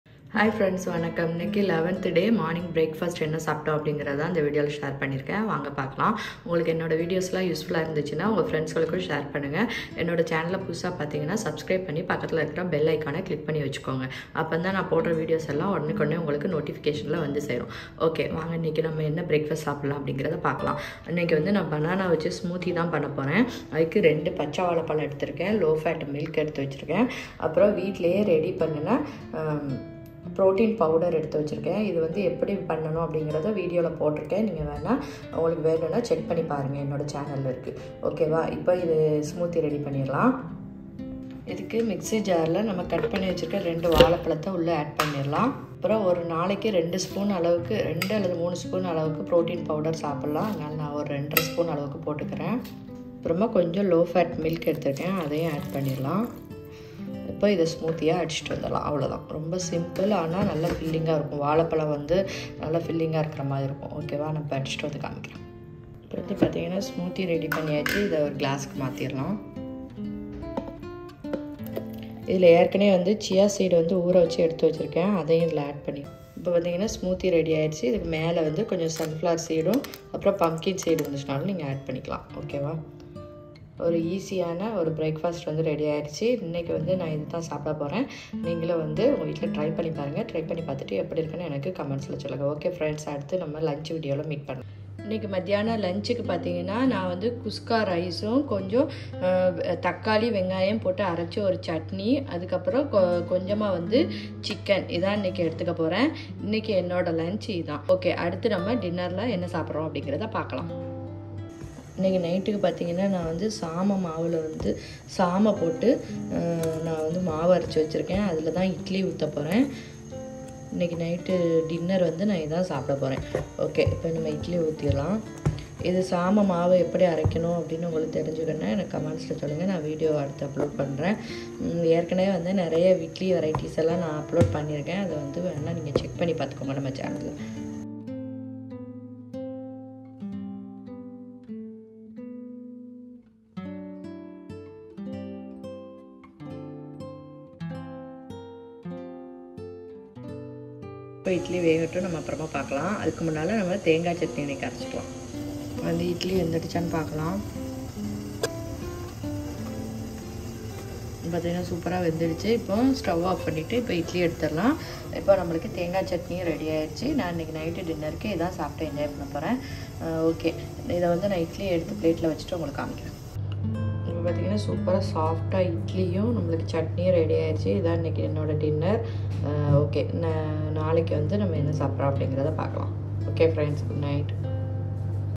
30 DAYS WEIGHTLOSS CHALLENGE || (30.12.2022) 11TH DAY DIET || BREAKFAST, LUNCH & DINNER
Weight loss recipes and ideas .